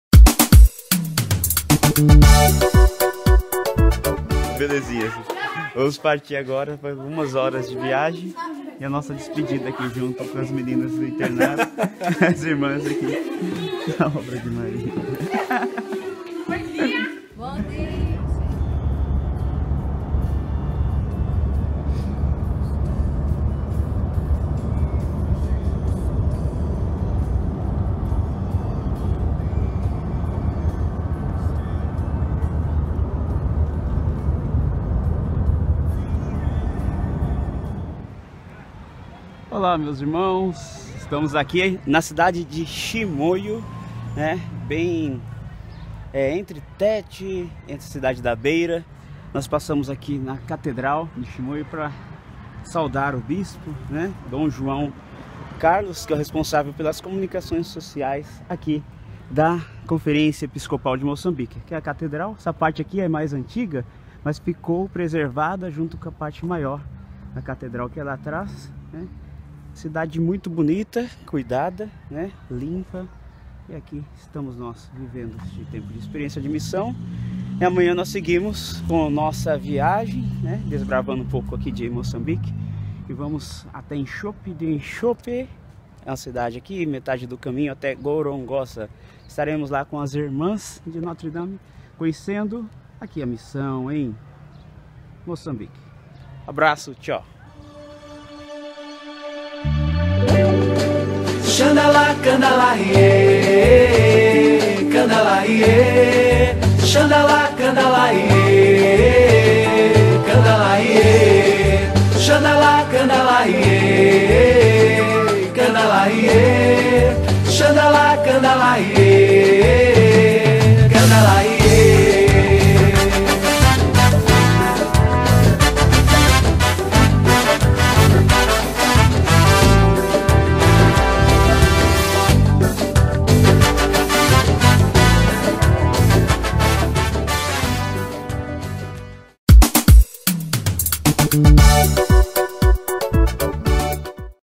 Encontro com o Bispo de Chimoio (Ep 59 - 2018)
Partimos de Dombe rumo a cidade de Chimoio, capital da província de Manica e sede da diocese de Chimoio para nos encontrar com o Bispo Dom João Carlos Hatoa. Ficamos alojados na Cúria Diocesana e no outro partimos para vila de Gorongosa, diocese da Beira em Sofala.